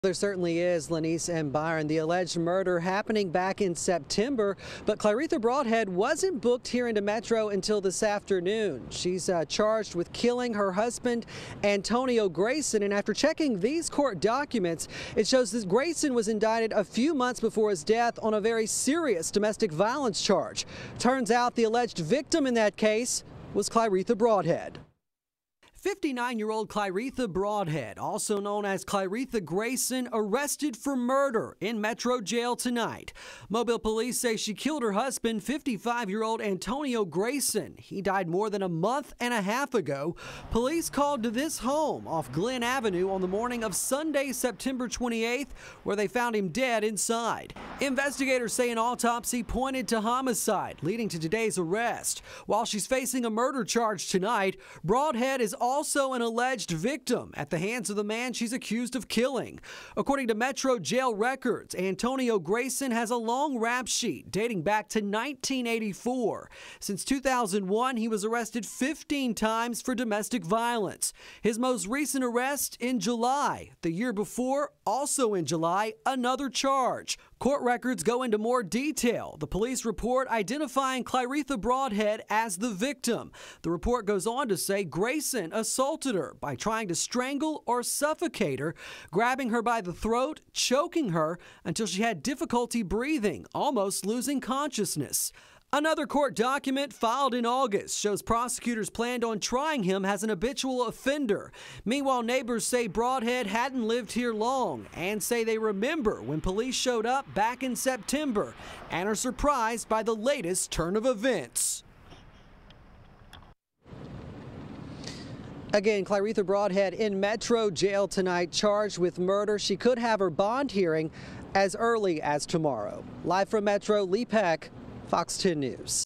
Man found dead in Midtown Mobile house identified; wife charged with murder
Detectives with the Mobile Police Department were called to a home Sunday, September 29 after a man was found dead.

Investigators said the man's body was discovered around 5 a.m. in a home on Glenn Avenue at Emogene Street.